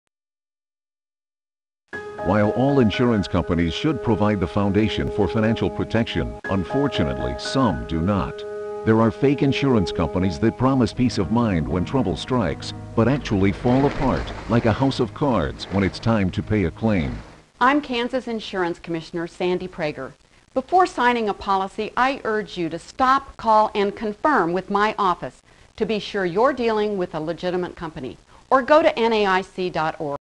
2007 House of Cards PSA
-- National Association of Insurance Commissioners (NAIC) public service announcement regarding fake insurance.